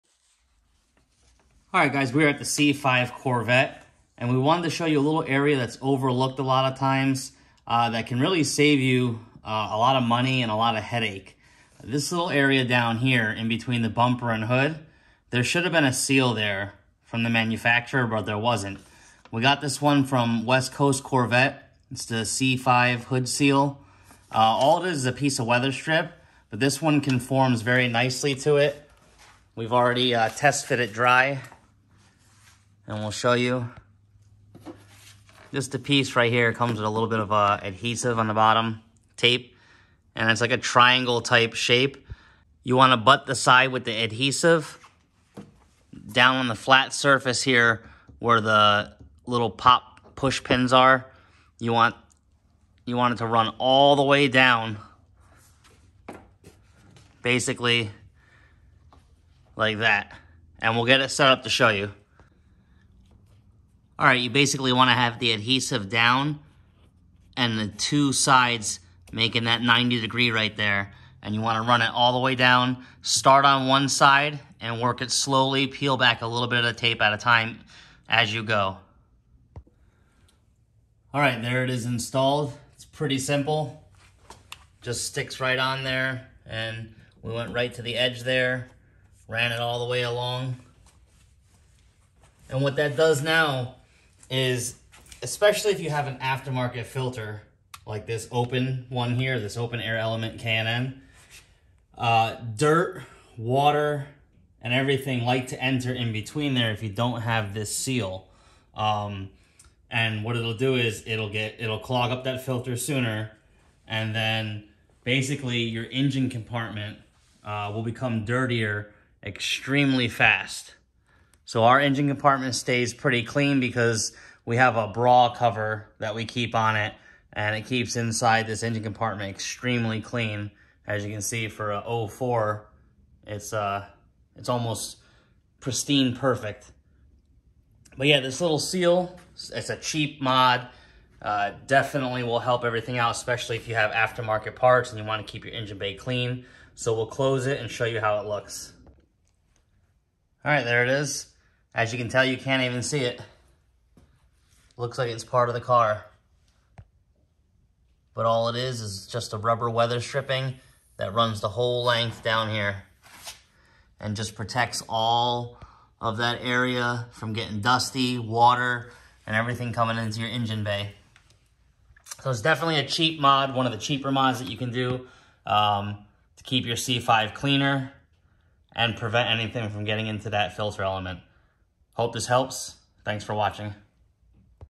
Cheap & EASY Mod to keep your C5 Engine bay & Open Element Air Filter CLEAN
This is a simple weather strip seal that should have come from the factory. It will keep dirt & water out of your C5's engine bay and air filter. Its a cheap mod that works!

Link for seal: